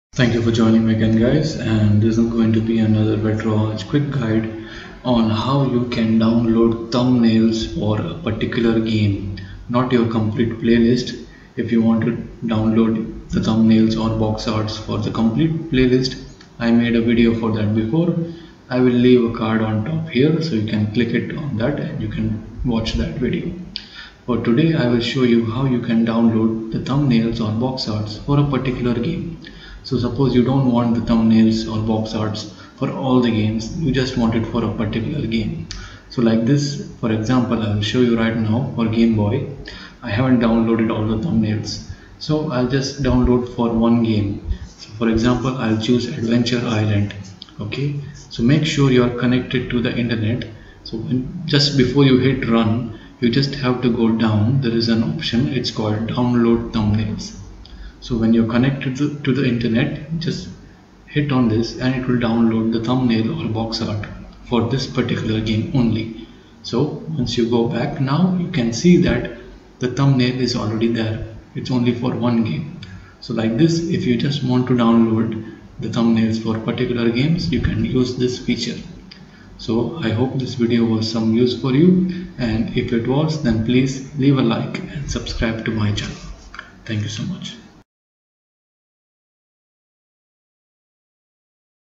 How To Download Thumbnails (Box art) One By One|Retroarch Quick Guide
In this video I will show you how to download a single thumbnail for a game, not the entire playlist in Retroarch.